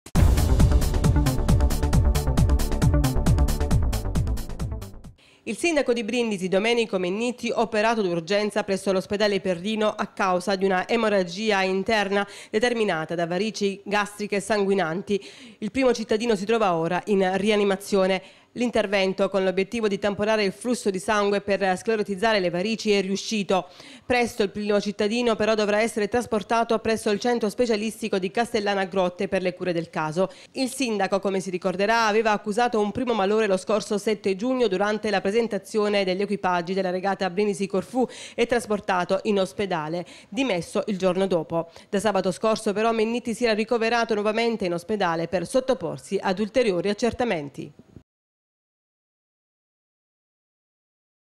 Brindisi Operato d'urgenza il Sindaco Domenico Mennitti
Brindisi- il Sindaco Domenico Mennitti è in osservazione presso l'ospedale "Perrino" di Brindisi a causa di complicanze determinate da "varici gastriche sanguinanti". Stamani i sanitari hanno eseguito un intervento-tampone per sclerotizzare tali varici, ma non escludono la possibilità di disporre un trasferimento immediato presso un Centro specializzato per le cure del caso.